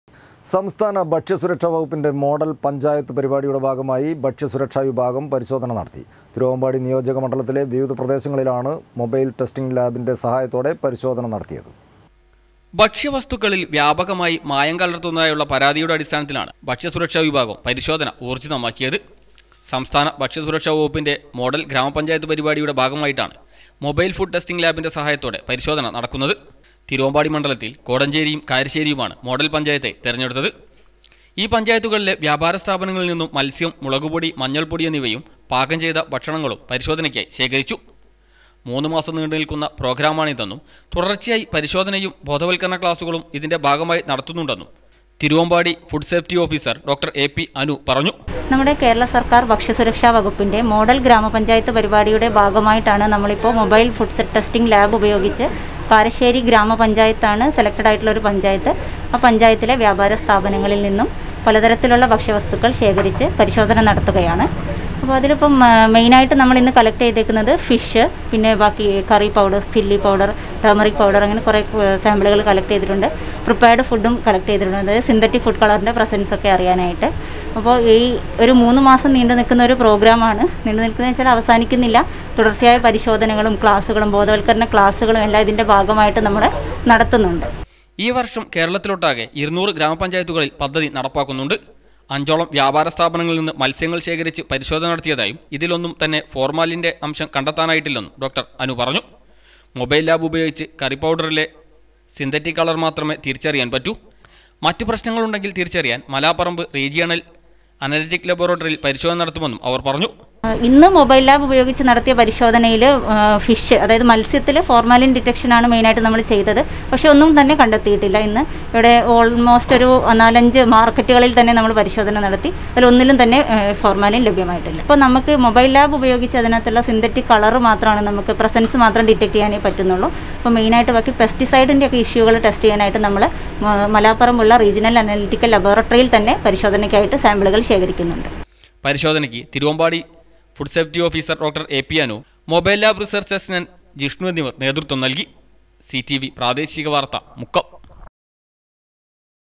സംസ്ഥാന ഭക്ഷ്യ സുരക്ഷ വകുപ്പിൻ്റെ മോഡൽ പഞ്ചായത്ത് പരിപാടിയുടെ ഭാഗമായി ഭക്ഷ്യ സുരക്ഷ
സംസ്ഥാന ഭക്ഷ്യ സുരക്ഷ വകുപ്പിൻ്റെ മോഡൽ പഞ്ചായത്ത് പരിപാടിയുടെ ഭാഗമായി ഭക്ഷ്യ സുരക്ഷ വിഭാഗം പരിശോധന നടത്തി. തിരുവമ്പാടി നിയോജക മണ്ഡലത്തിലെ വിവിധ പ്രദേശങ്ങളിലാണ് മൊബൈൽ ടെസ്റ്റിംഗ് ലാമ്പിൻ്റെ സഹായത്തോടെ പരിശോധന നടത്തിയത്